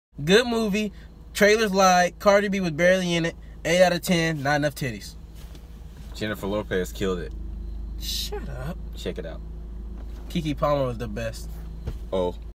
World's Shortest Hustlers Movie Review
Short on time but wanna know if Hustlers is worth seeing, this the video for you. Straight to the answer spoiler free review.